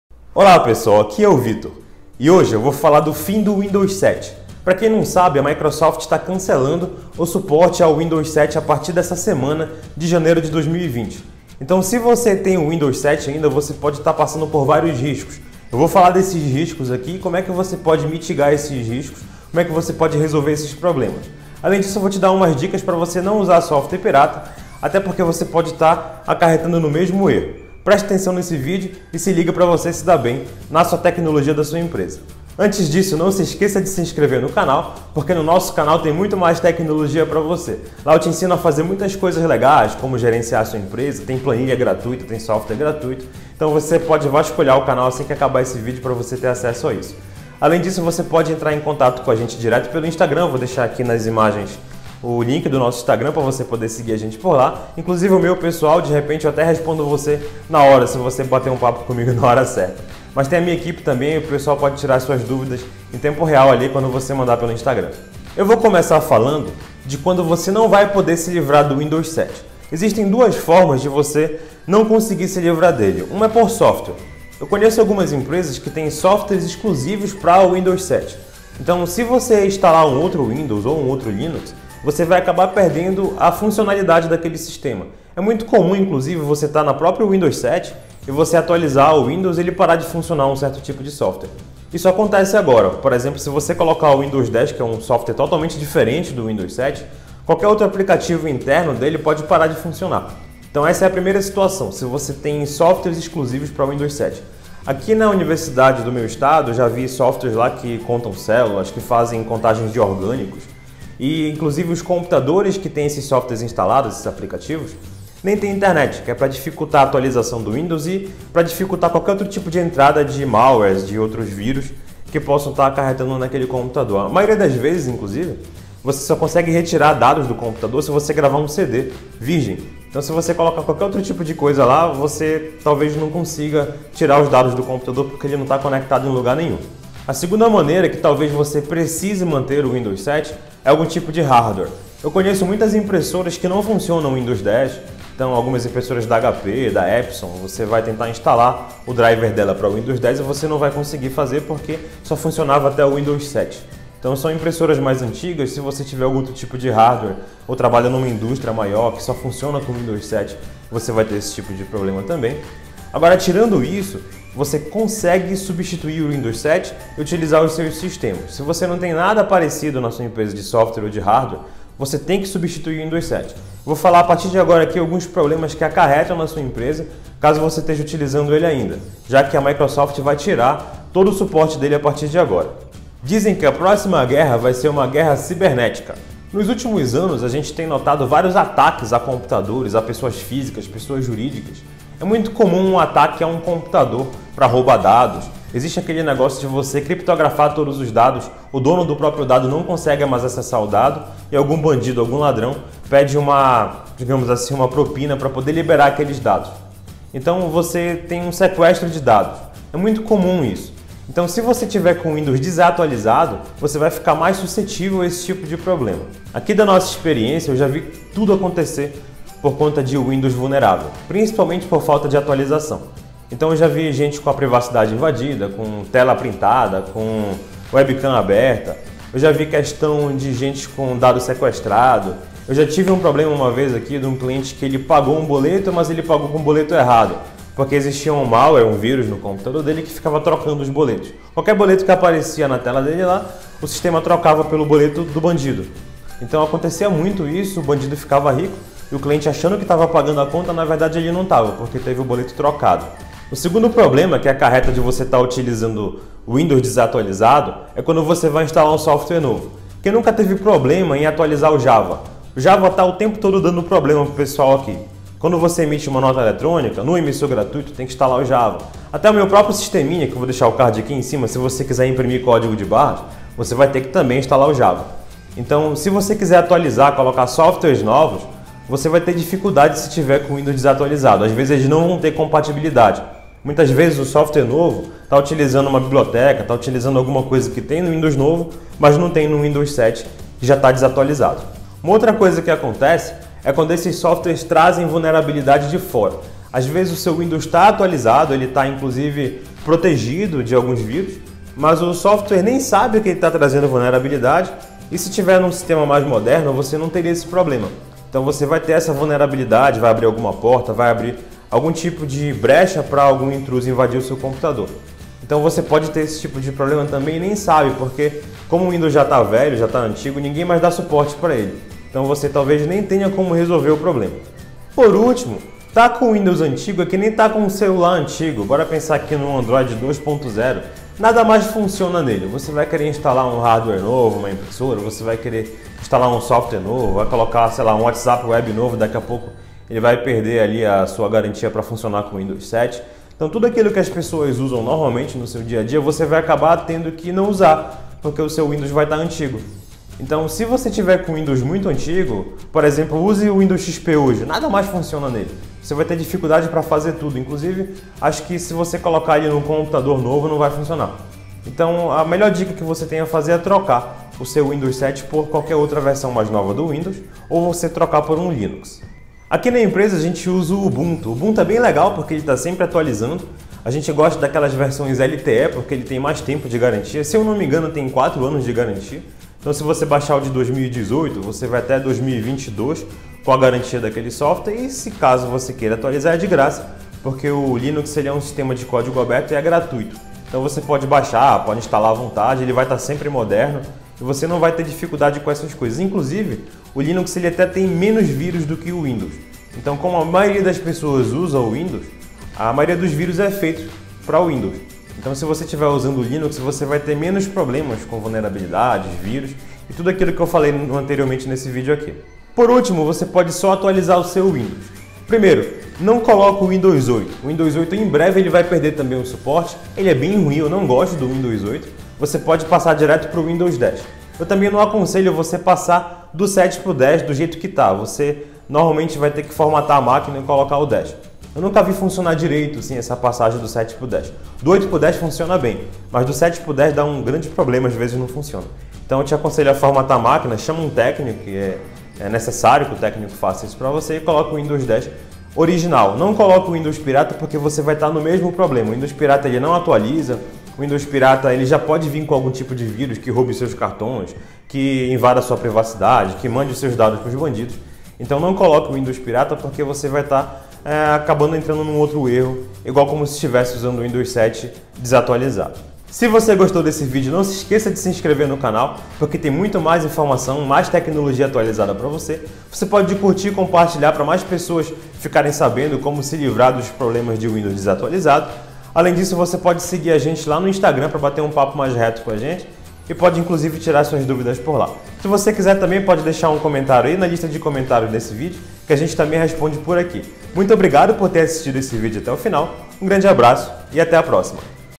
FIM do Suporte ao Windows 7 - Cuidado com o que pode acontecer
Windows 7 NÃO VAI PARAR DE FUNCIONAR, mas você precisa ter cuidado com ele. Nesse vídeo, eu te ajudo com os cuidados que você precisa ter com o Windows 7.